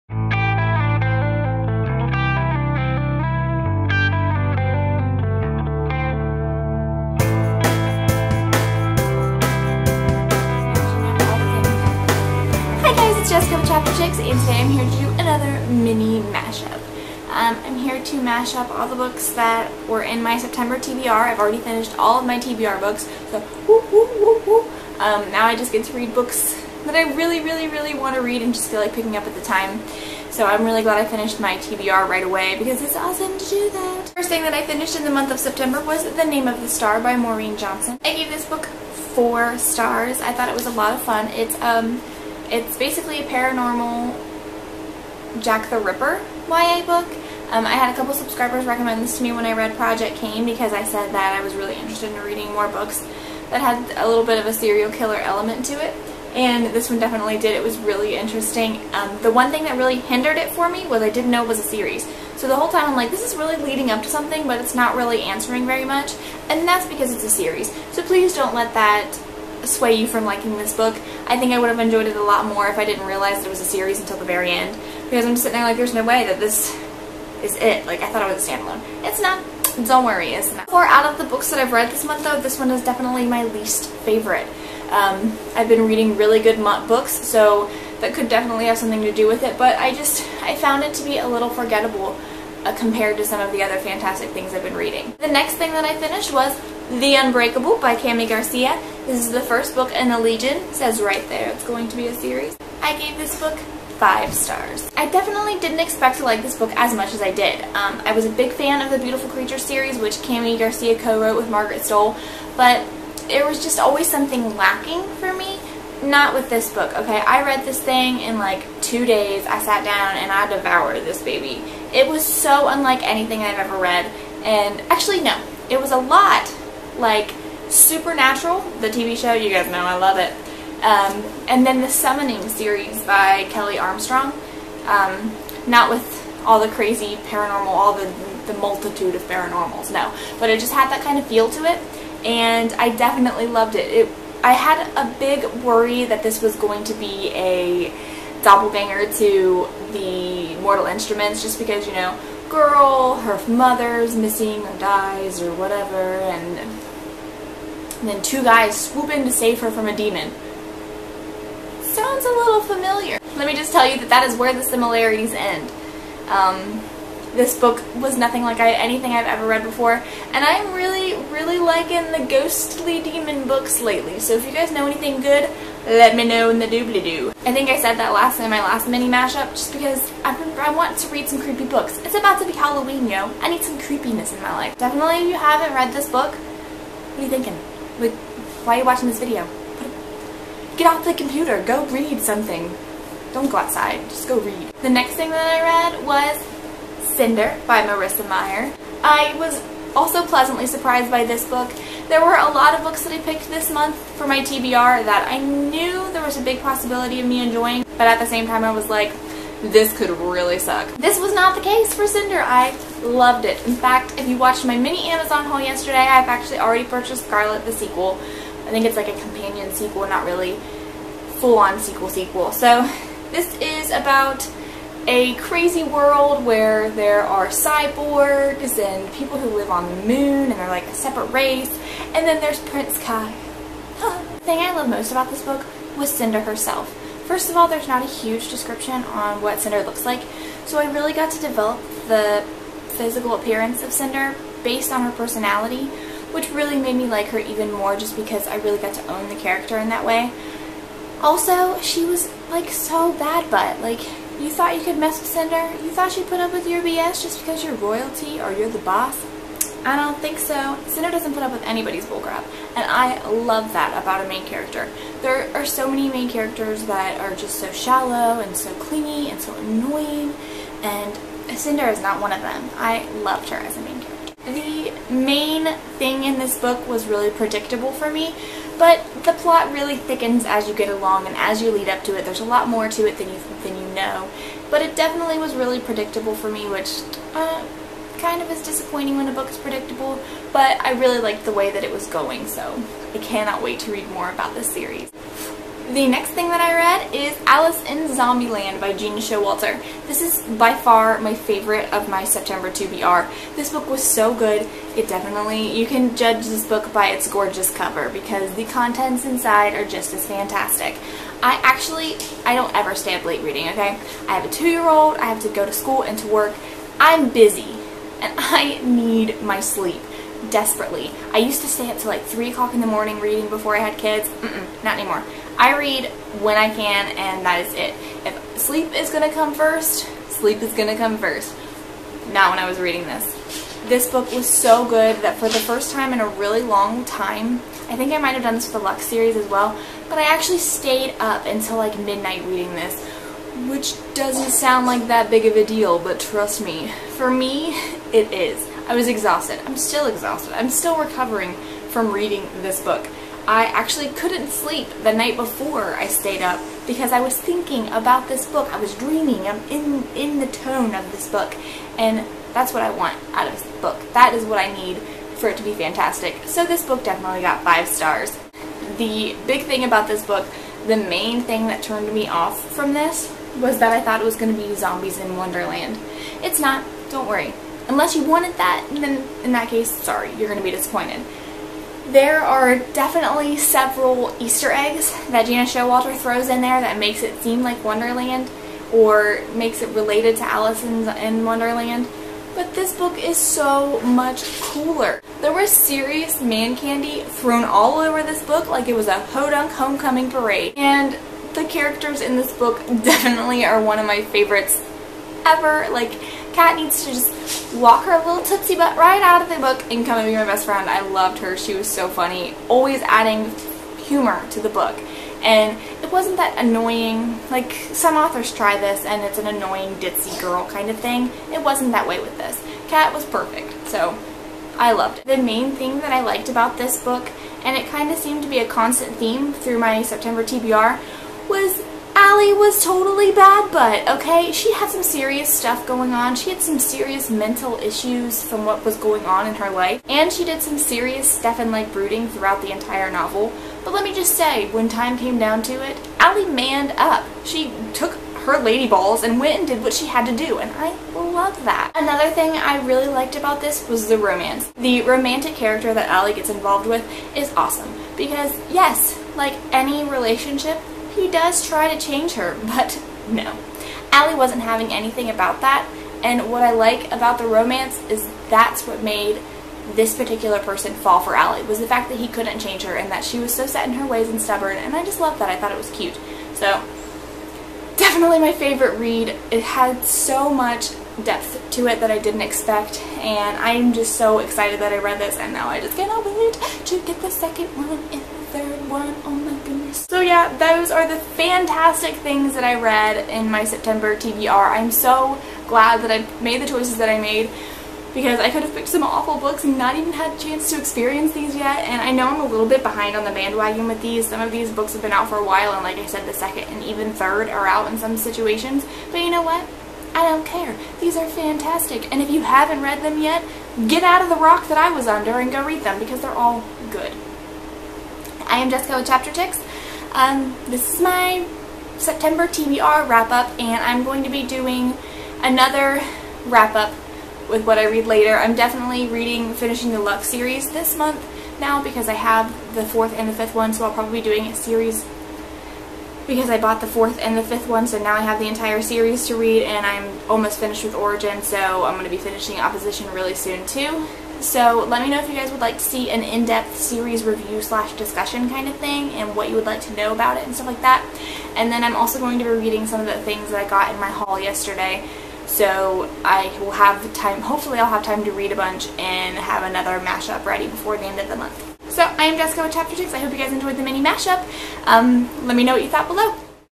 Mash-Up Book Reviews
I am reviewing all books I included in my September TBR video!

Name of the Star by Maureen Johnson
Unbreakable by Kami Garcia
Cinder by Marissa Meyer
Alice in Zombieland by Gena Showalter